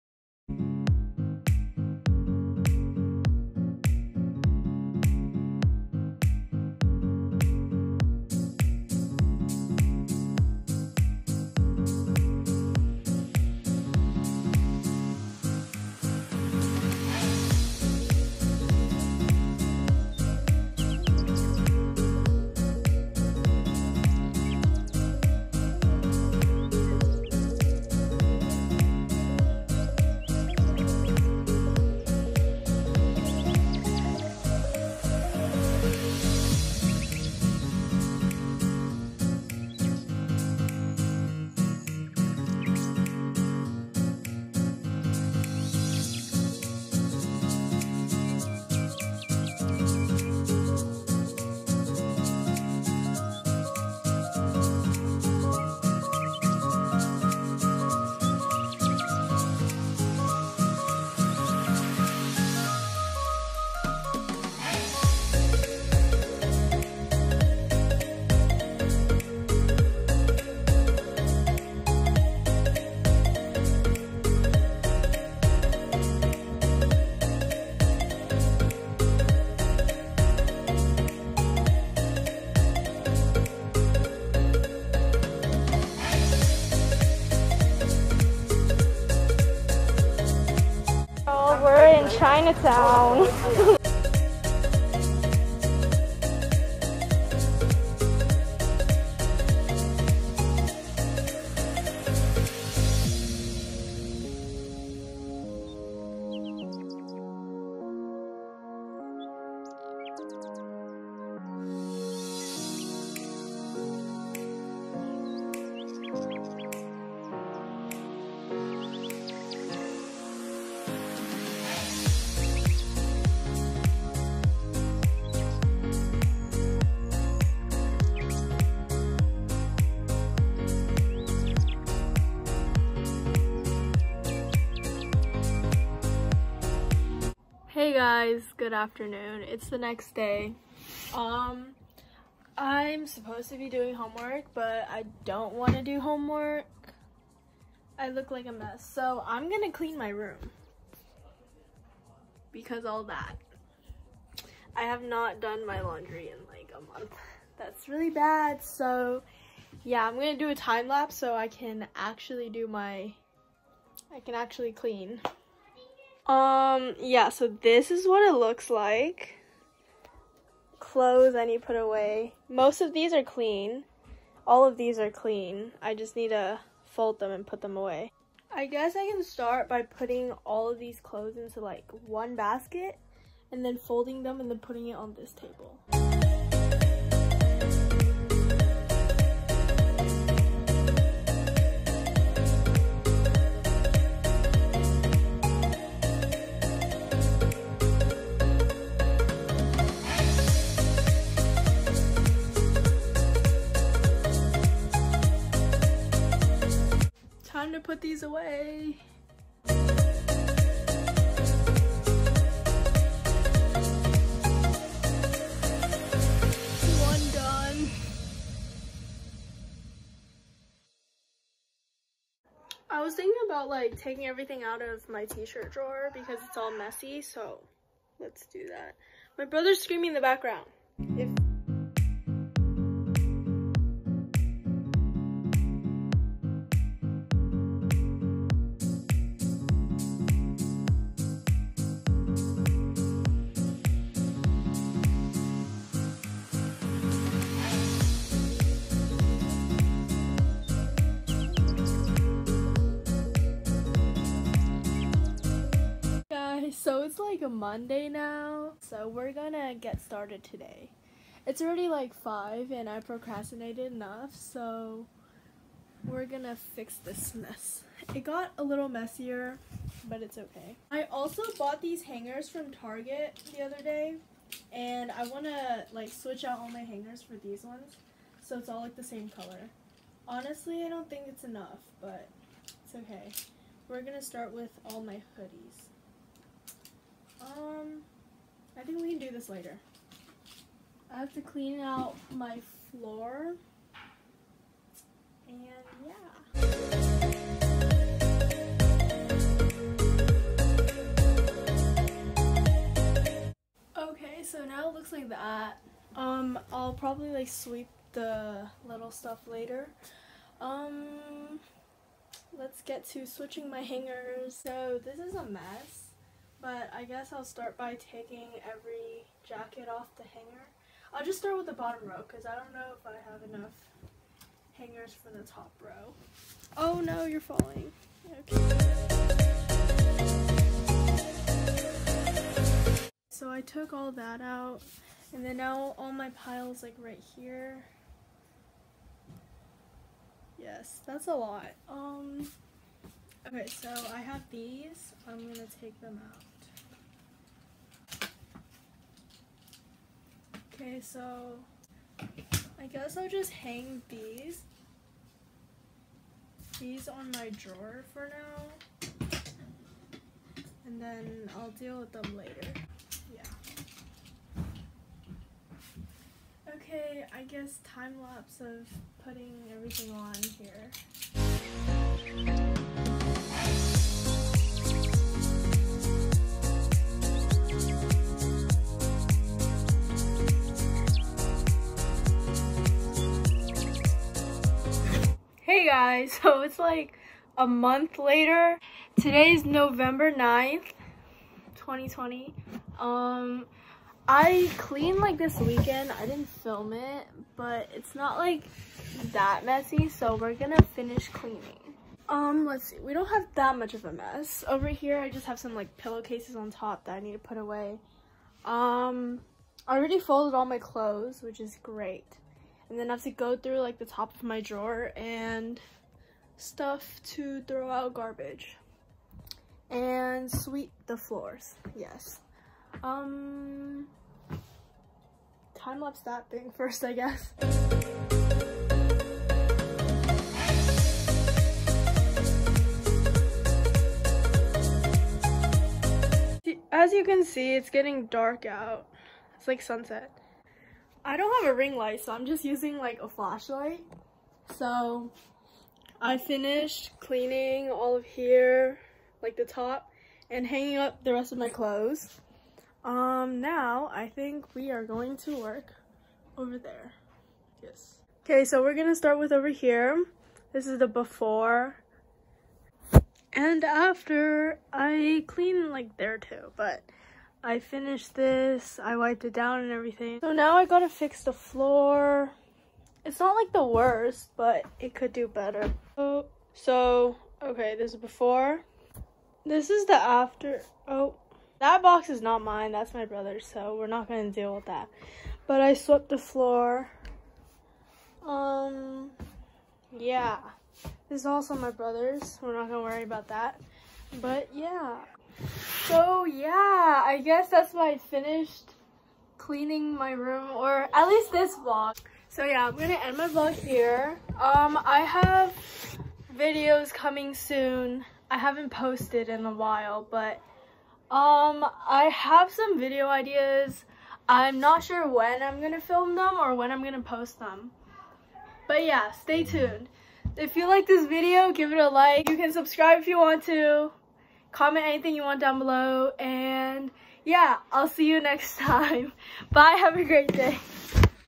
VLOG! SF adventures and cleaning my room...again lol
HI guys! Sorry I haven't posted in a while but we're back with another vlog! We went to SF and had some fun (and food) and we gotta clean my messy room again haha.

ABOUT ME:
Hi there! I'm Phoebe and I LOVE photography, editing, and sports.